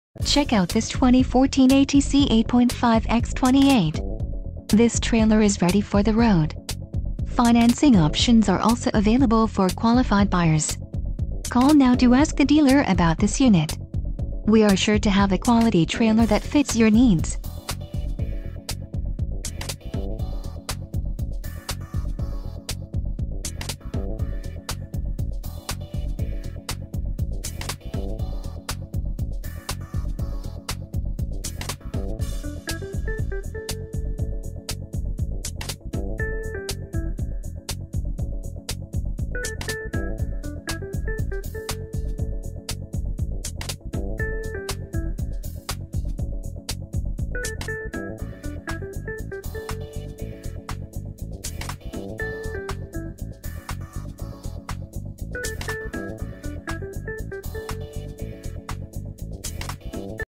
2014 ATC 8.5 x 28
" 12 Inch Extended Tongue, 48 Inch Side Door, 36in x 36in Side Hinged Generator Door, 48 Inch Fluorescent Light, Aluminum Wheels, 7 Ft Tall, Ramp, Cabinets, 2 - 5200 lb. Axles"